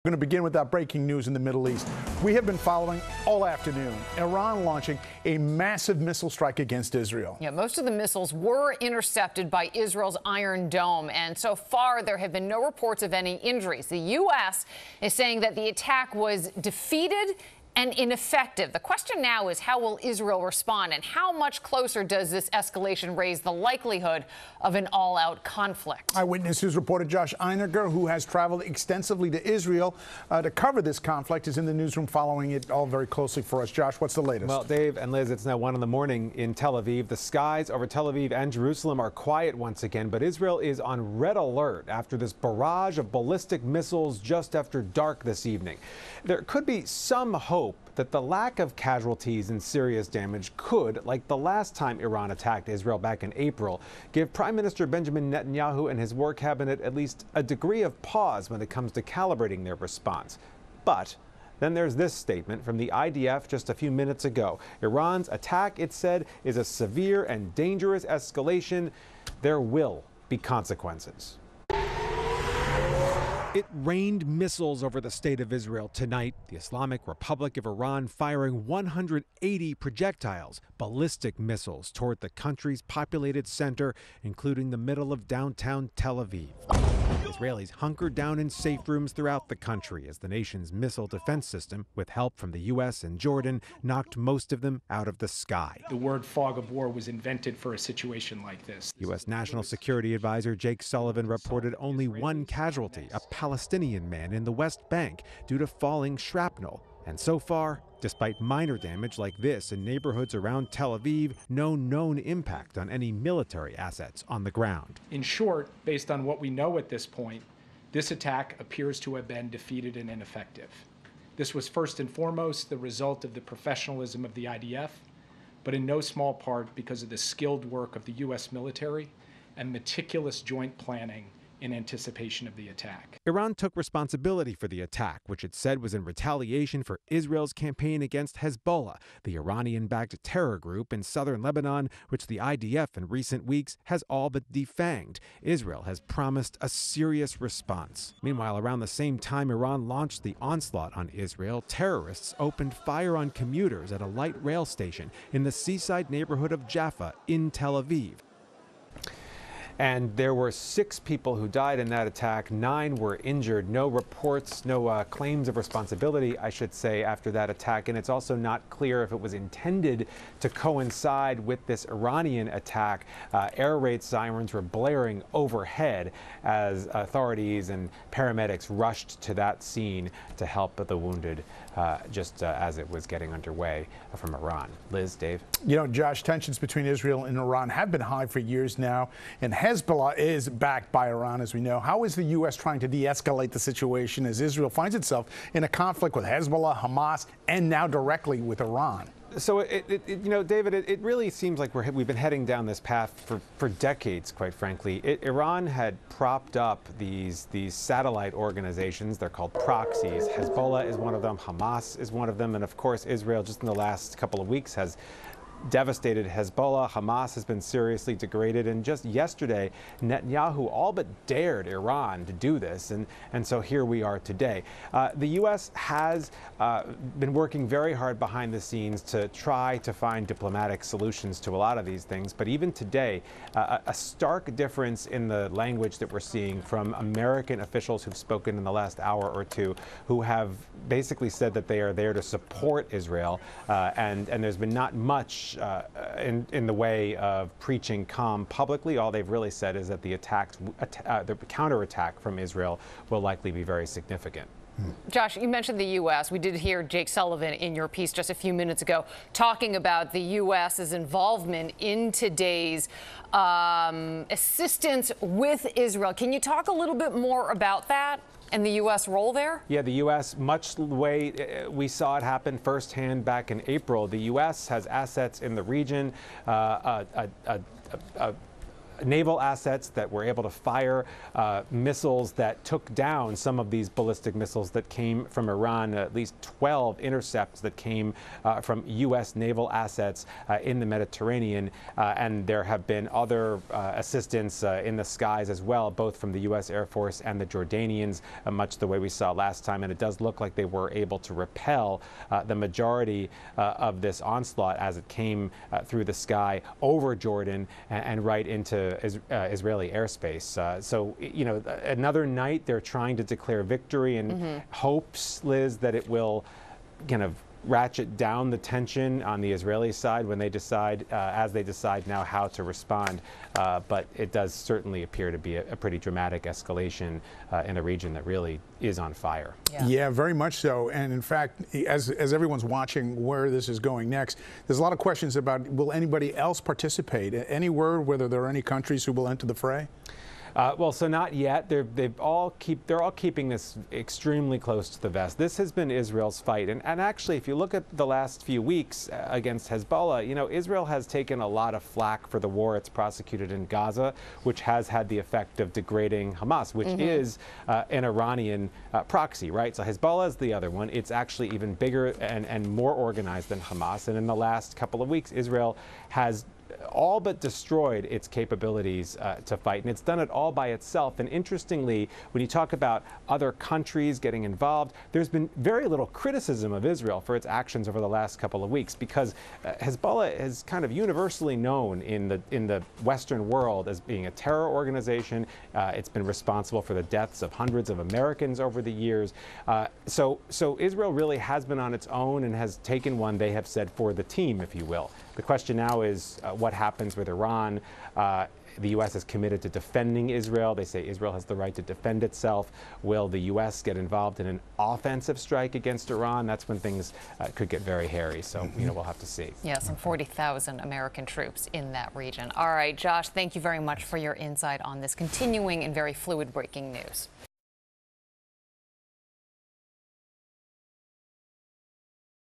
Iran launches missile attack on Israel
Josh Einiger has the latest.